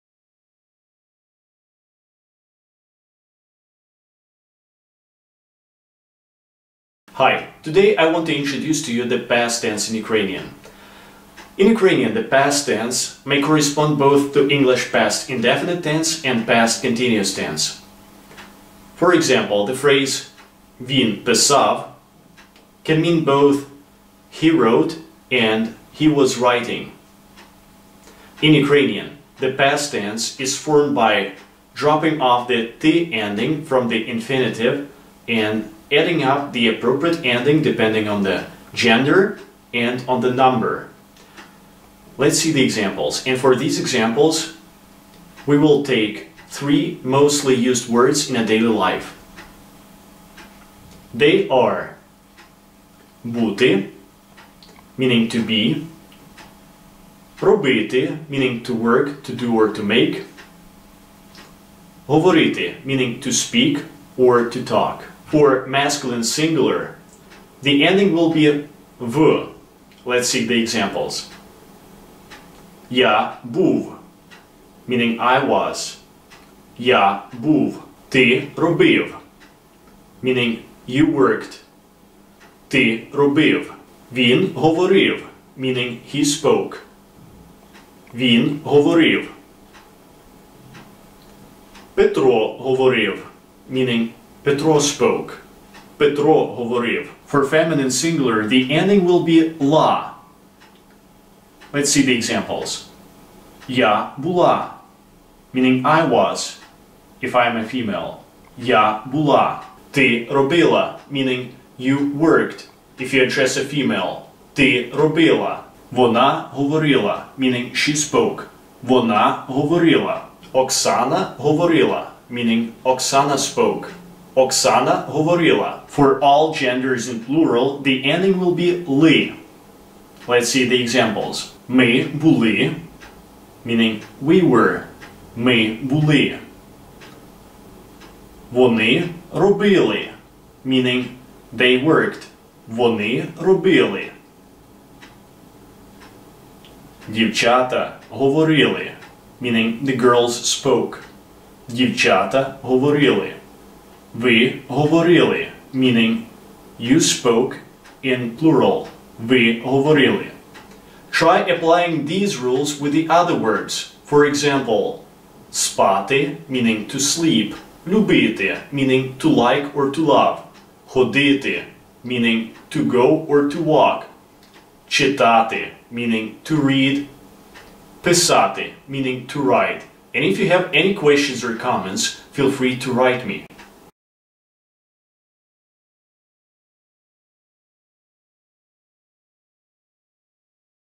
Learn Ukrainian. Intermediate Level: Lesson 2
Ukrainian Grammar: Ukrainian Past Tense.
Past Tense in Ukrainian